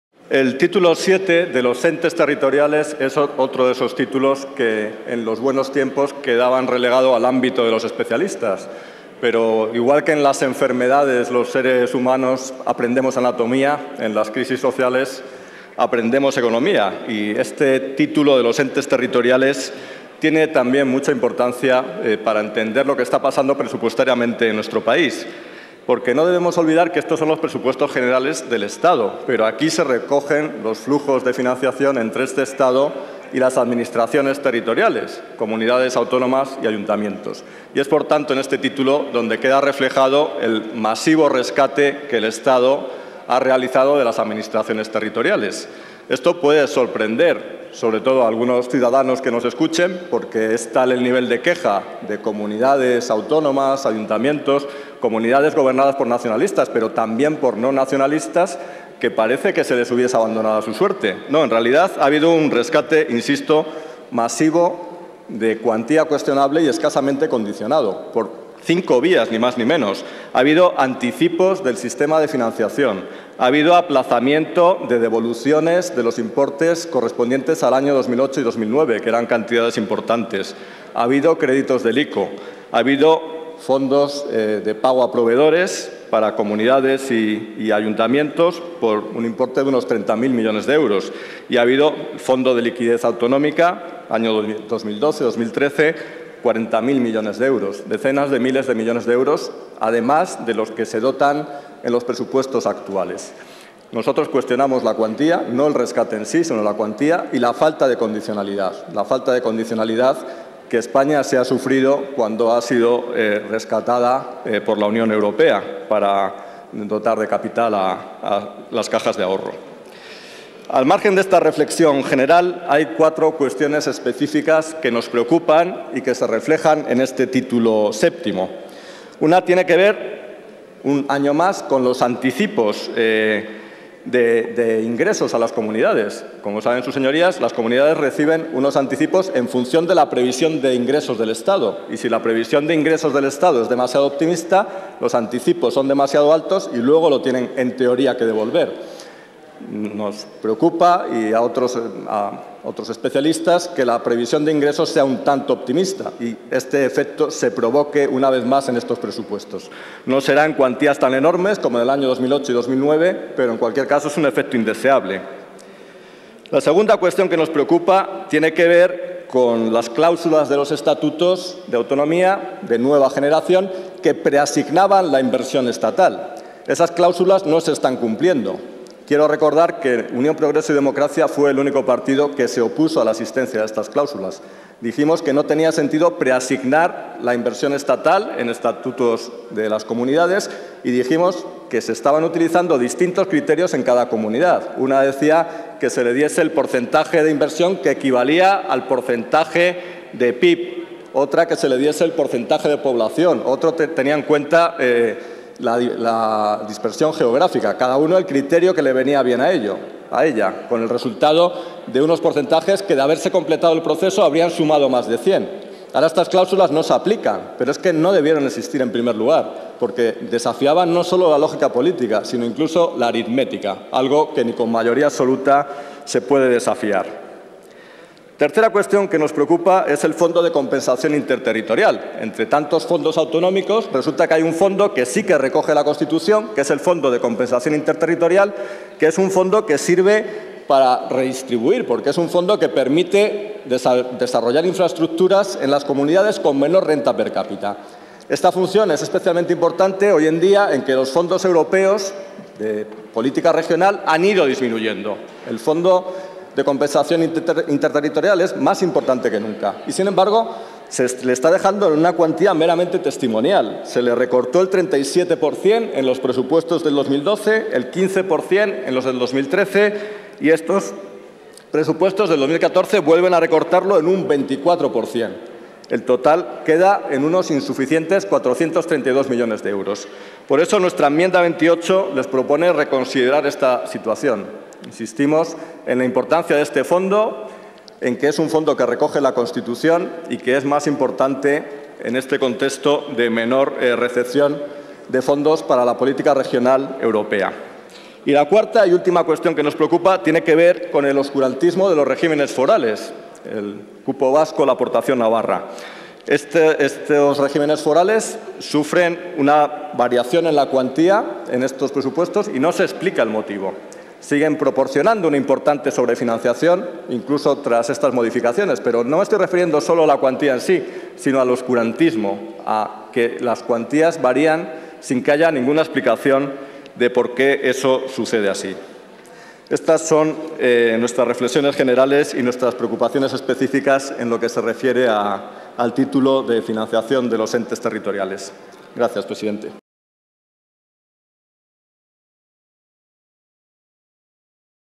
Anchuelo (UPyD): "Continúa en estos presupuestos el oscurantismo de los regímenes forales"
El diputado magenta critica, con respecto al rescate masivo a la comunidades autónomas, la cuantía y la falta de condicionalidad, no el rescate en sí.

Anchuelo propone crear una comisión de estudio de los Fondo de Compensación Interterritorial a lo largo del 2014 para evaluar su funcionamiento de forma que a partir del año 2016 mejore su eficiencia y se adecue su dotación a las necesidades reales de nuestro país y poder así cumplir con el mandato otorgado a este fondo en la Constitución.

Considera que habría que hacer énfasis en el compromiso con el cumplimiento de los objetivos de déficit y por tanto el ánimo de responsabilidad presupuestaria al no proponer un aumento este año como se ha venido haciendo en el pasado sino una revisión de este sistema y dejando clara la necesidad de aumentar estos fondos en el futuro para que cumplan la función que se les asignó en la constitución de una manera eficaz.

Hace mención también al oscurantismo de los regímenes forales, ya que sus cuantías varían sin que haya ninguna explicación.

Intervención de Álvaro Anchuelo, UPyD, sobre el dictamen de comisión sobre el Proyecto de Ley de Presupuestos Generales del Estado para el año 2014, Título VII, entes territoriales.

Pleno 11/11/2013